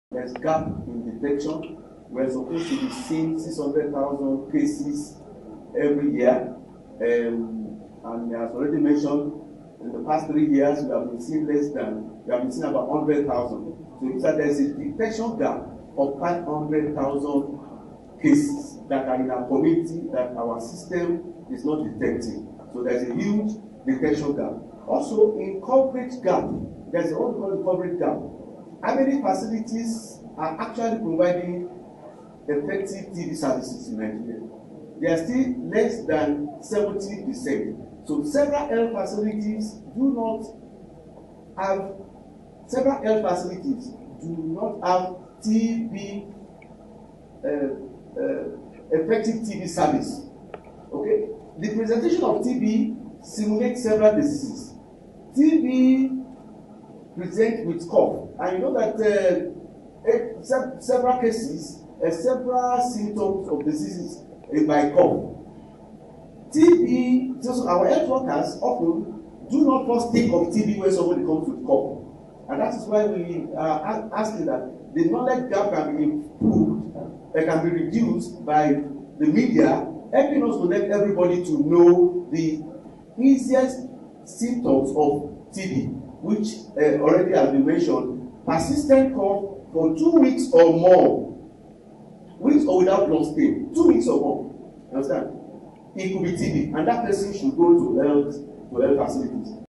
Ayodele Awe, of WHO, says half a million TB cases go undetected in Nigeria
Up to 500,000 cases of tuberculosis go undetected each year in Nigeria, creating a gap that could potentially threaten control of the disease across Africa. By Judd-Leonard Okafor